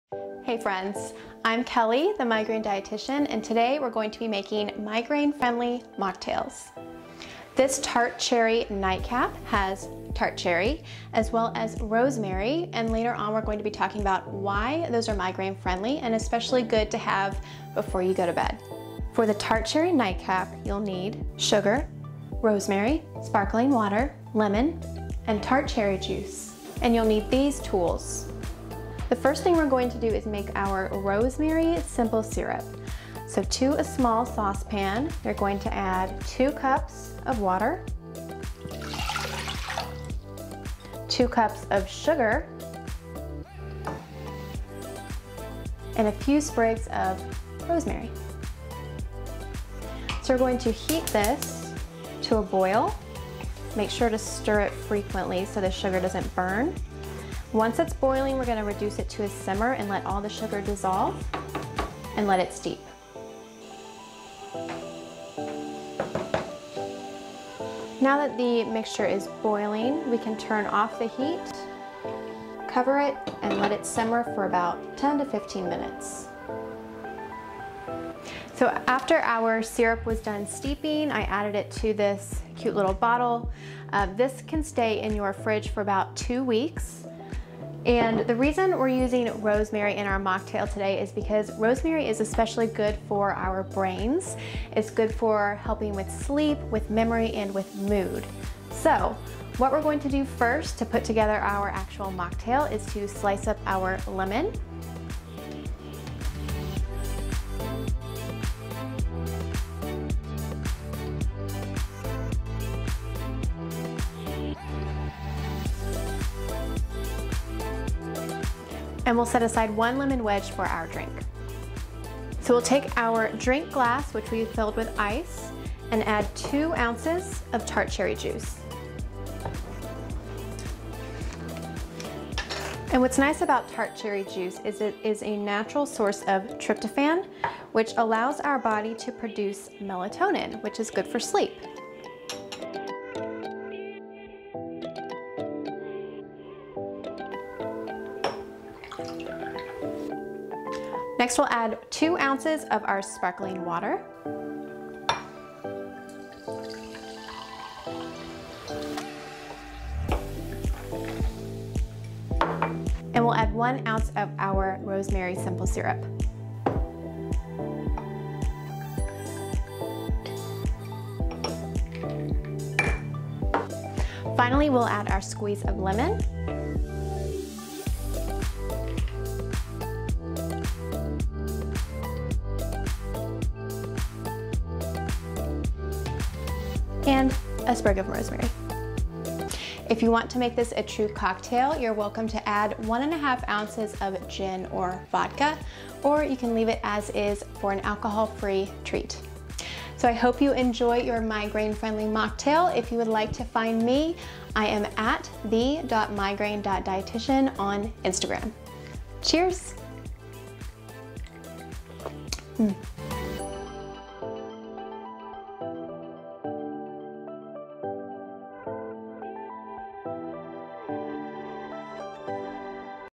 Holiday Mocktails - Tart Cherry Nightcap with The Migraine Dietitian | Cove
Looking for a migraine-friendly cocktail to celebrate the holiday season? Check out The Migraine Dietitian's Tart Cherry Nightcap recipe.

At Cove, we want to help you better manage your migraines. Want to see what it’s all about?